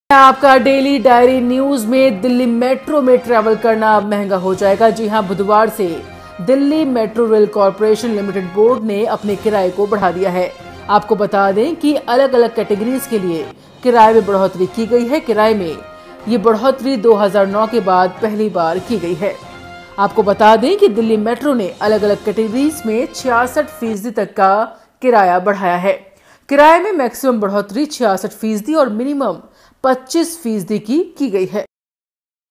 दिल्ली मेट्रो का सफ़र हुआ महँगा |delhi metro fare increase
दिल्ली मेट्रो में सफर करना हुआ महंगा, 8 से बढ़कर 10 रुपये हुआ न्‍यूनतम किराया